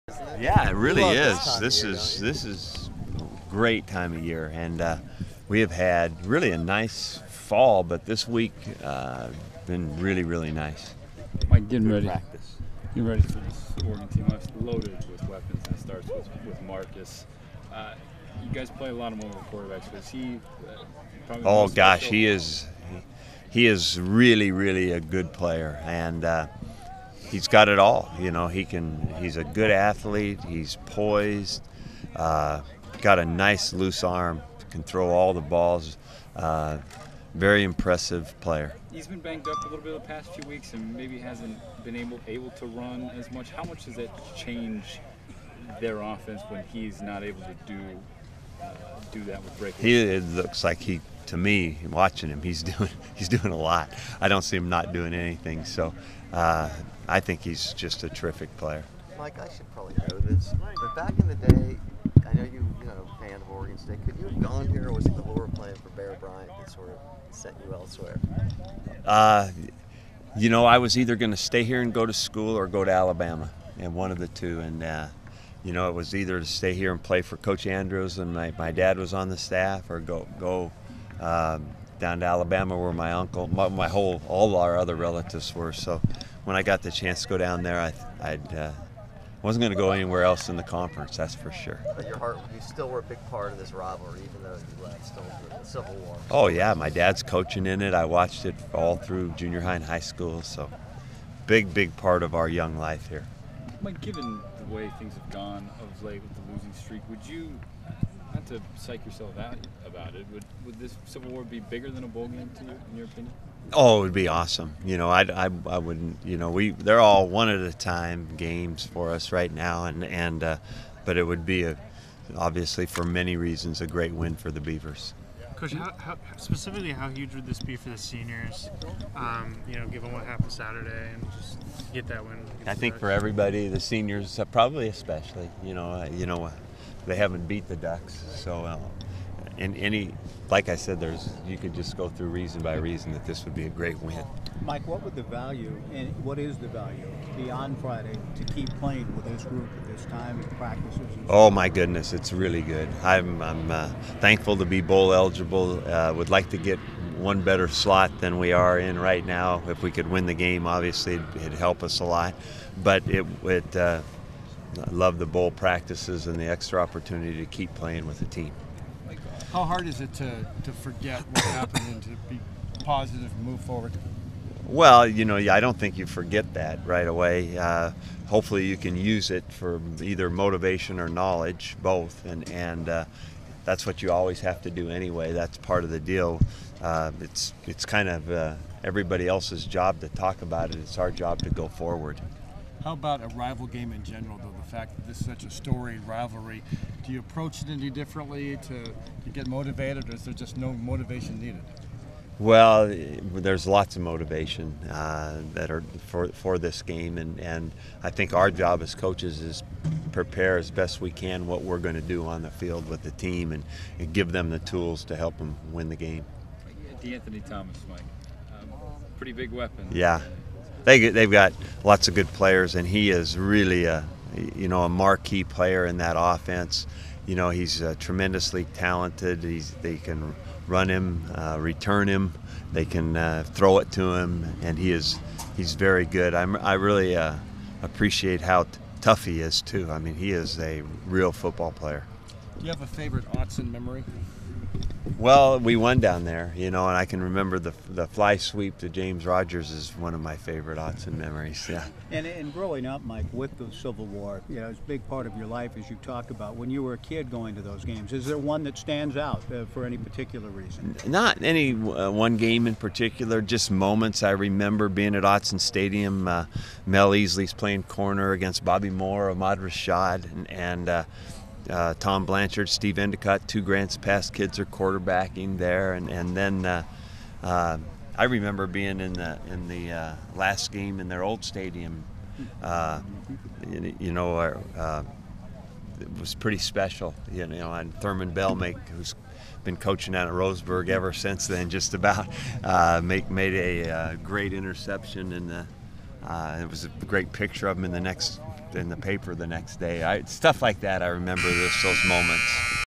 Mike Riley Civil War Week Post-Practice (11/26/13)
Oregon State head coach Mike Riley talks to the media as the Beavers prep for the 2013 Civil War in Eugene against the Ducks.